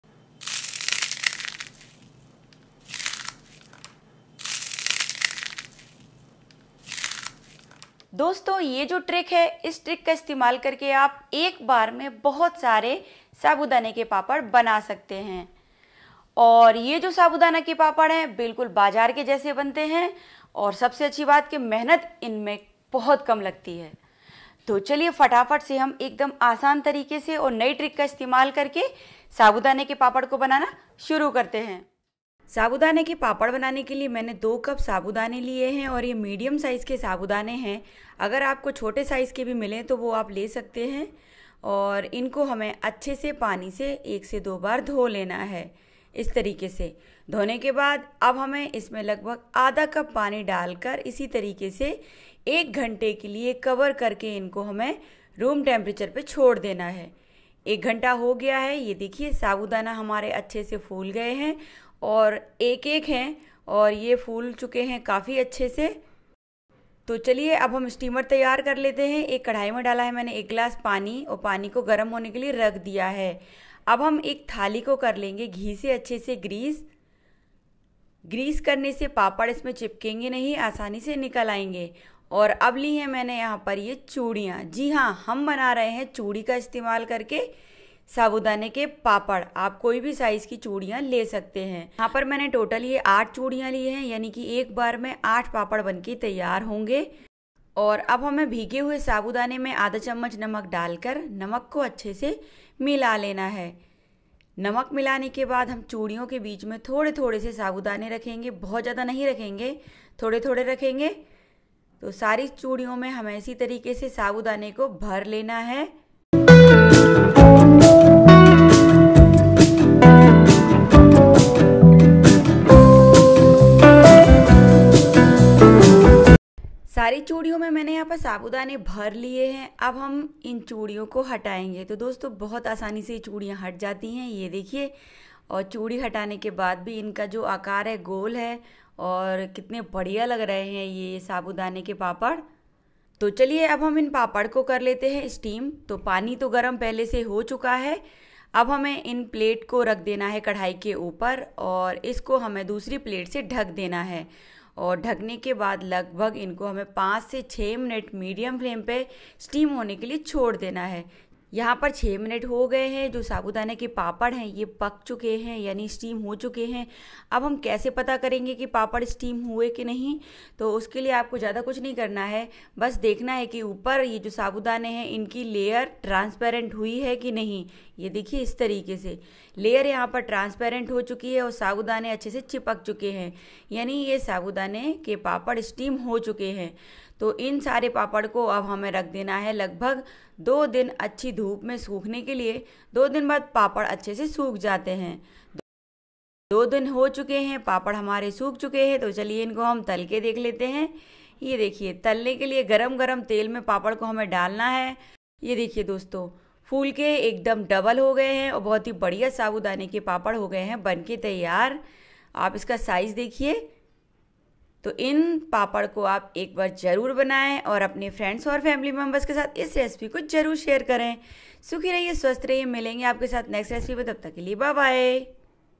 एक दम नयी ट्रिक से मिनटों मे बनाये बाजार से भी अच्छे साबूदाना पापड़ वो भी चूड़ी से/Sabudana Papad
आज मे एक दम आसान तरीके से एक बार मे बहुत सारे साबूदाना के पापड़ बनाना बता रही हूँ ये पापड़ बहुत ही ज्यादा जल्दी बनते है और बनाने मे मेहनत भी बहुत ही ज्यादा कम लगती है
INGREDIENTS
2 cup sabudana
1/2 cup water
1/2 tbsp salt
SabudanaPapad,# SagoPapad